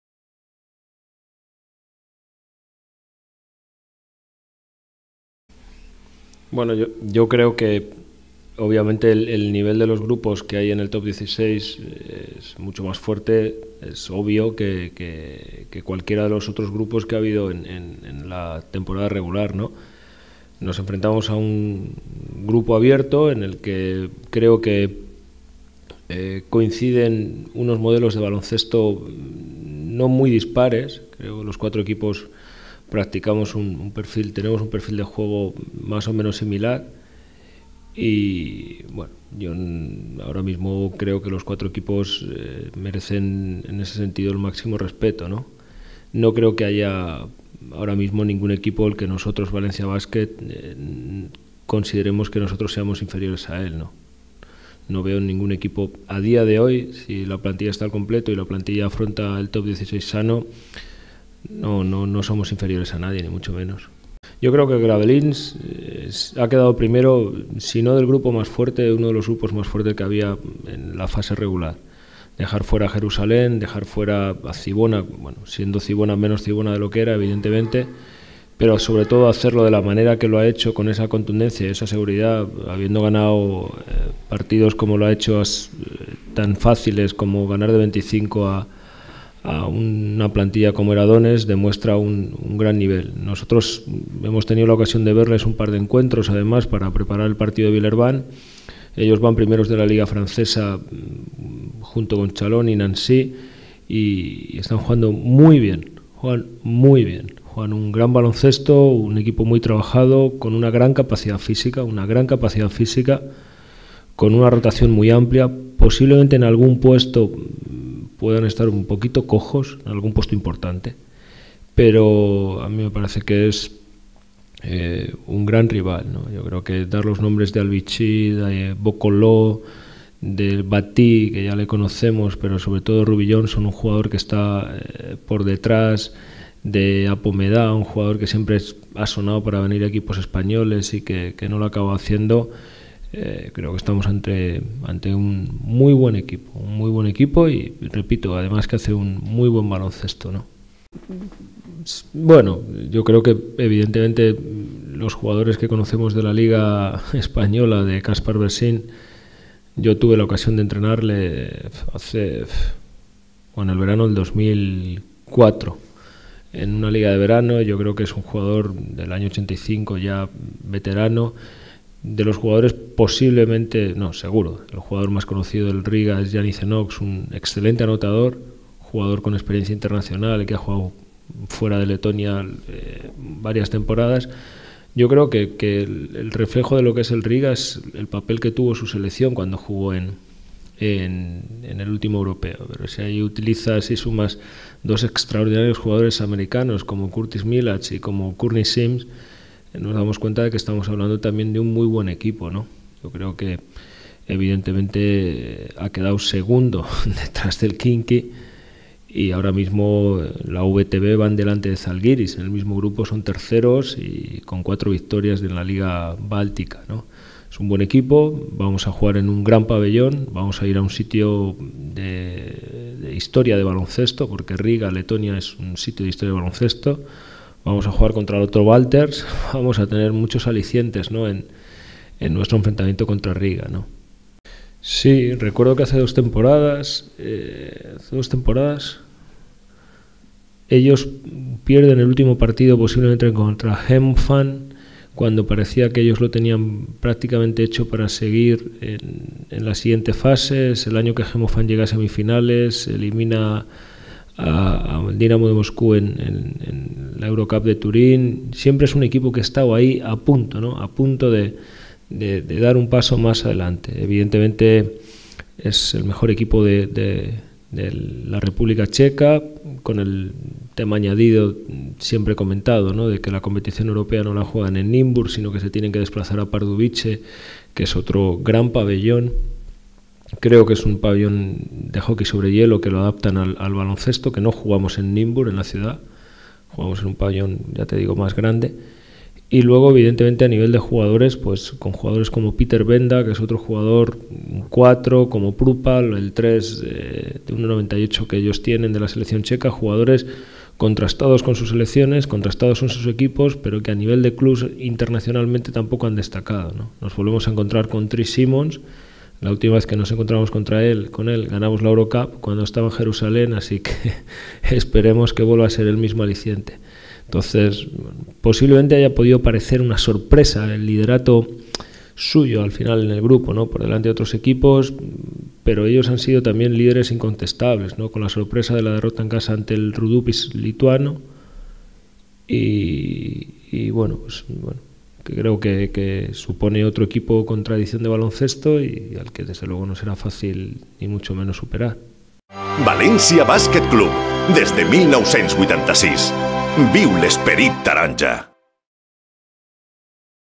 Chechu Mulero, el Last16
El segundo entrenador del Valencia Basket analiza el grupo y a cada uno de los rivales en el Last16